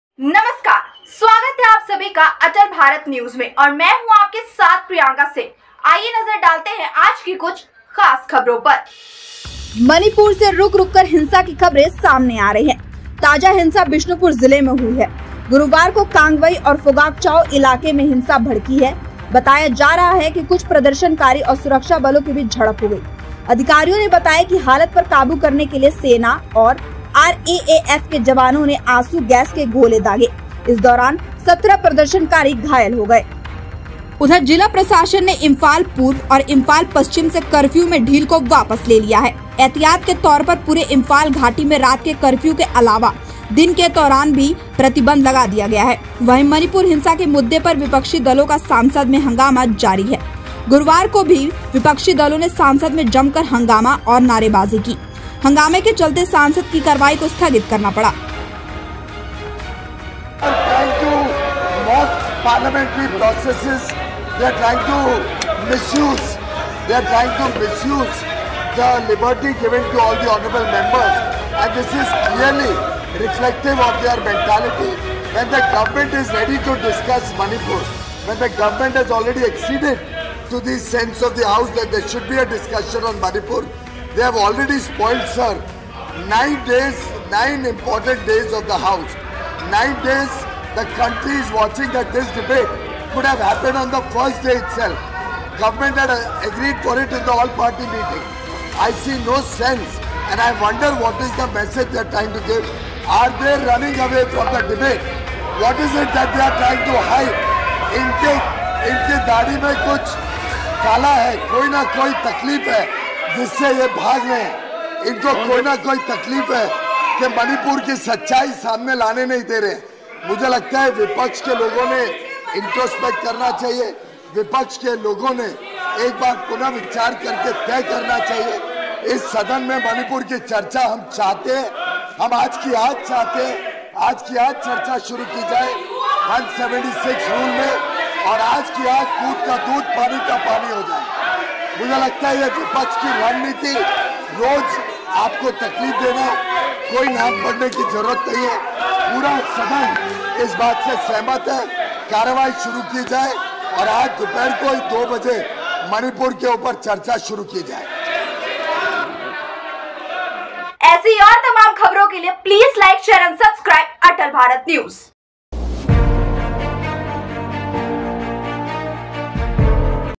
मणिपुर में फिर भड़की हिंसा, विष्णुपुर में हिंसक झड़प में 17 लोग घायल, सुरक्षा कर्मियो पर किया पथराव
मणिपुर में फिर भड़की हिंसा, विष्णुपुर में हिंसक झड़प में 17 लोग हुए घायल, मैतेई समुदाय ने सुरक्षा कर्मियों पर किया पथराव, देखें।।